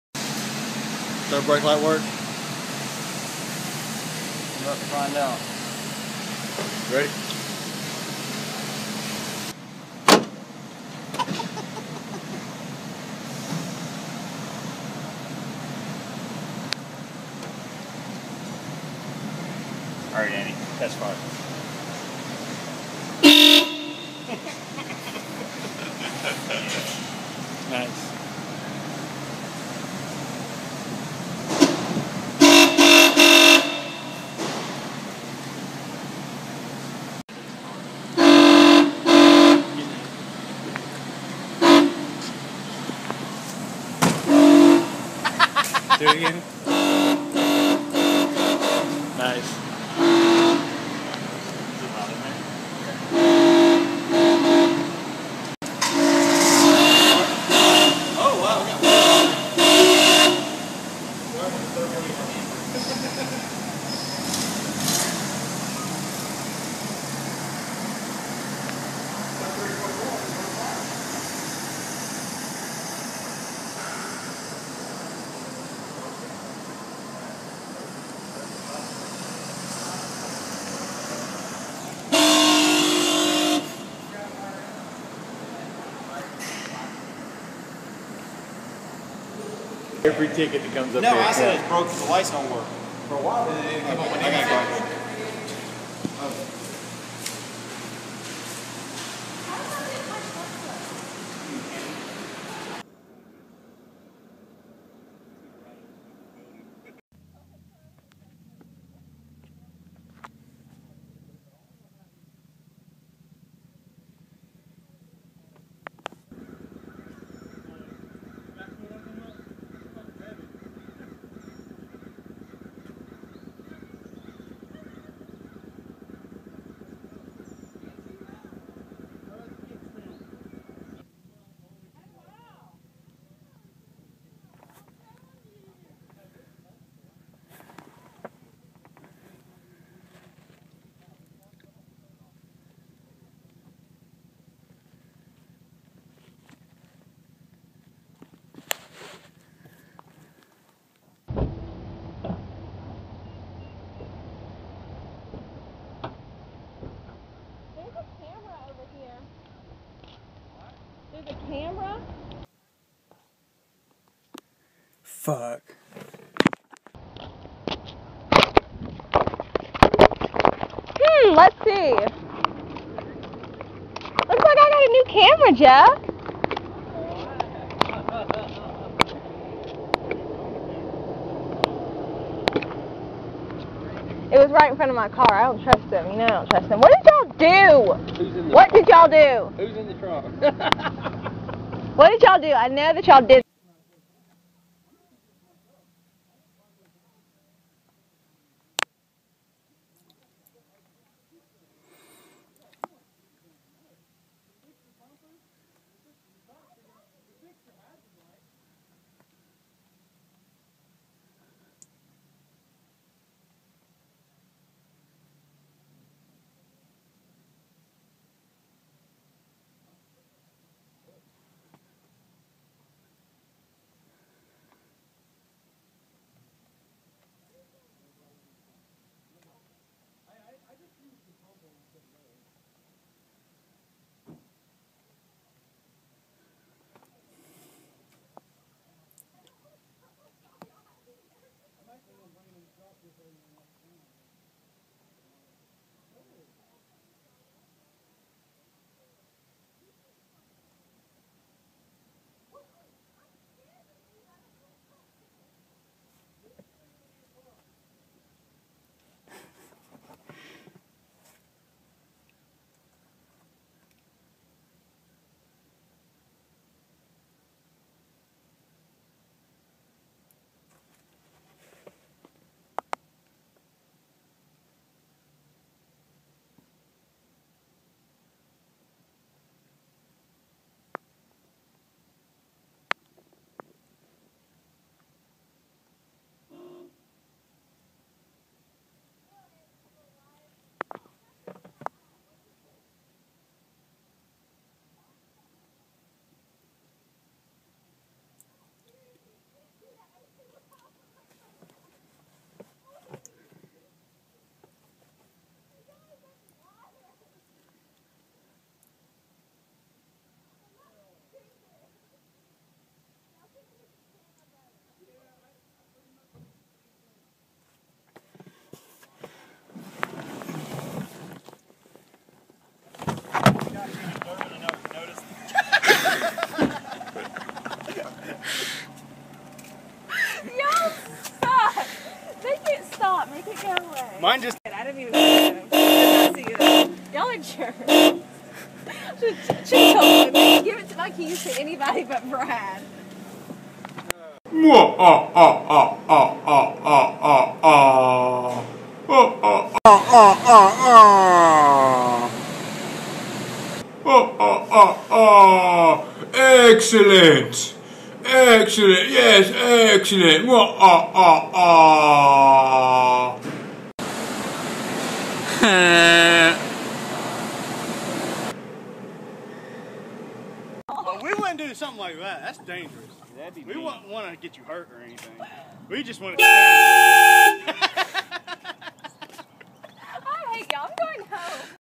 May 21st Prank
Prank:  Car horns hooked up to brake lights.

May 21st:  Victoria Day